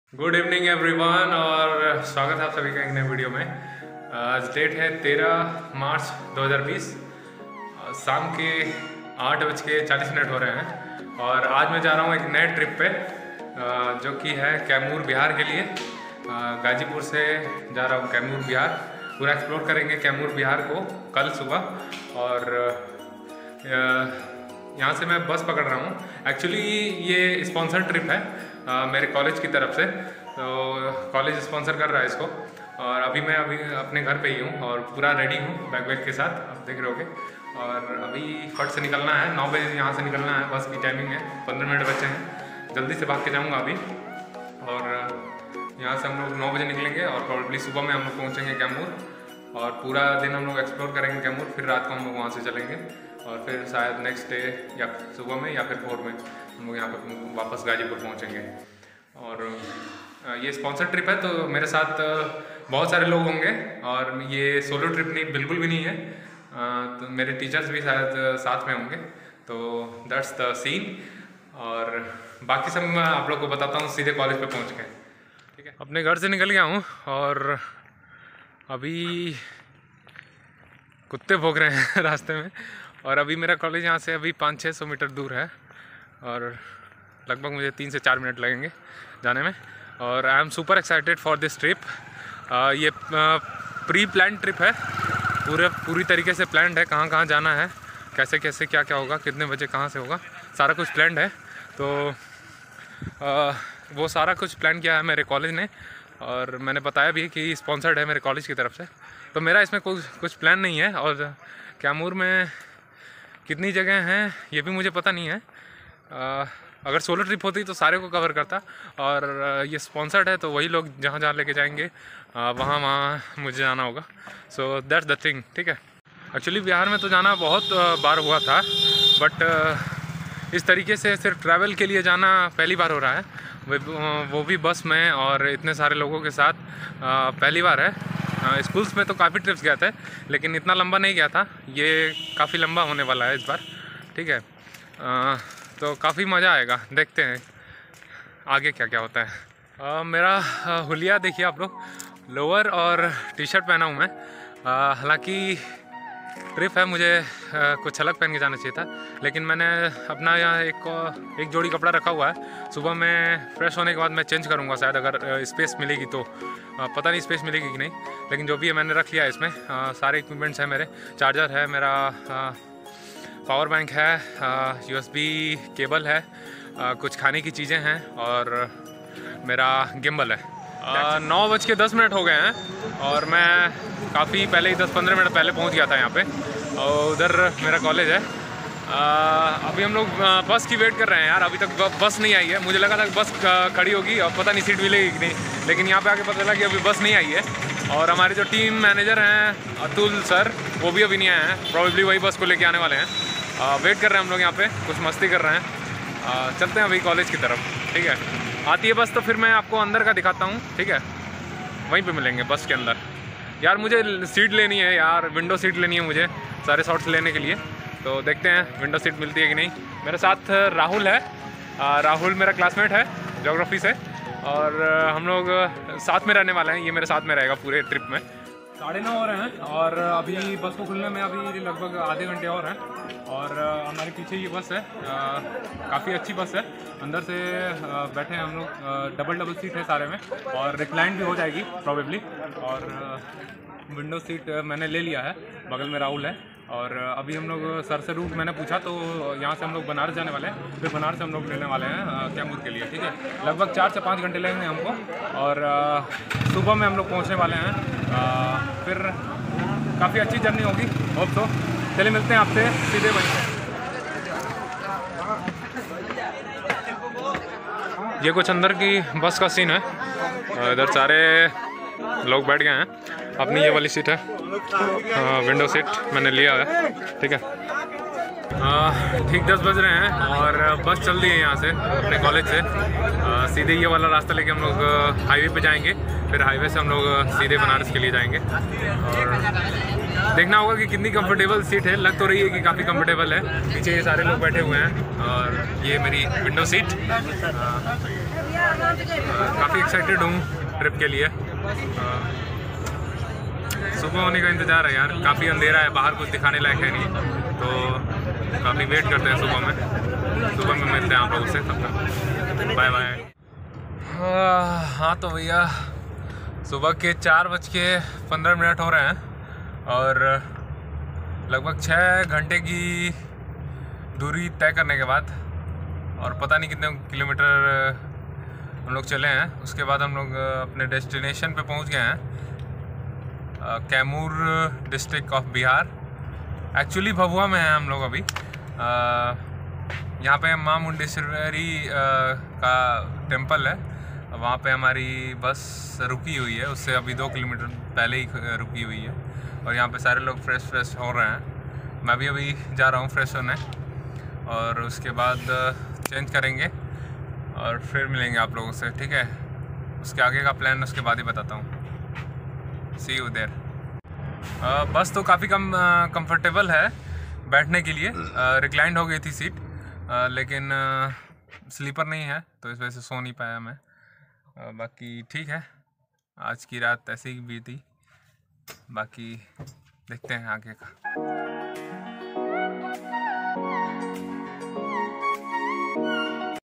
My College Trip To Kaimur Bihar | Vlog #1 | Explorebug
Hello Everyone ,
This was a sponsored and organized trip by my college (Pg College Ghazipur) in which we were traveling to Kaimur, Bihar from Ghazipur, UP.

Note - This channel and I do not claim any right over any of the musics used in this video.
                     Musics Credited